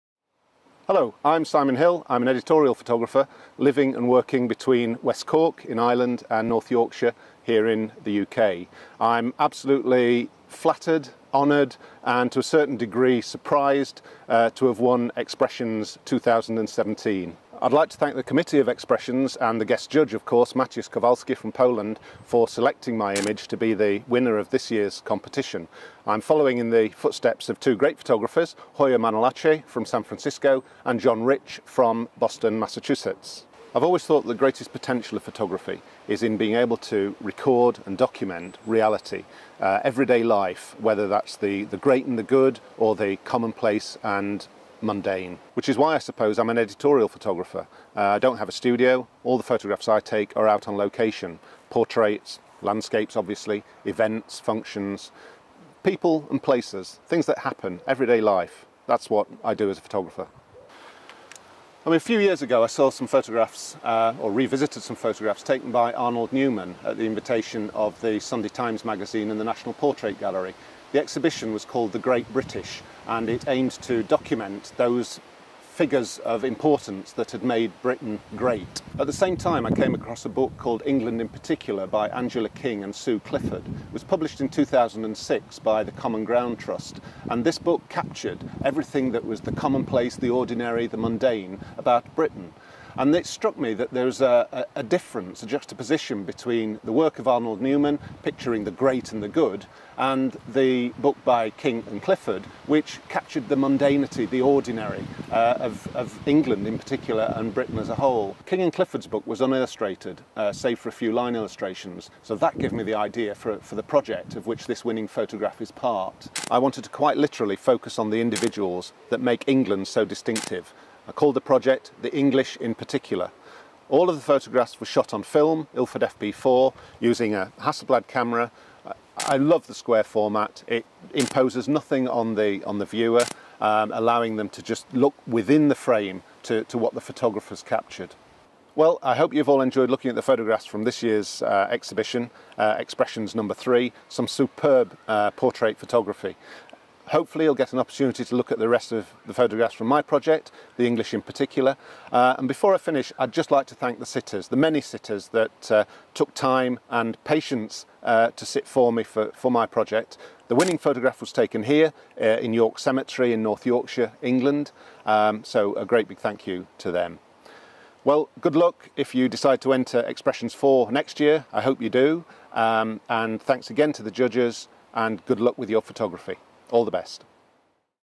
Simon Hill, winner Longford digital Arts Expressions 3 2017
Video of Photographer Simon Hill, winner of the Longford Digital Arts Expressions 3 portrait competition.
In this video Simon talks about the inspiration for his winning entry.

The video was shot on location in York Cemetery on a JVC 4K video camera and edited in Final Cut Pro X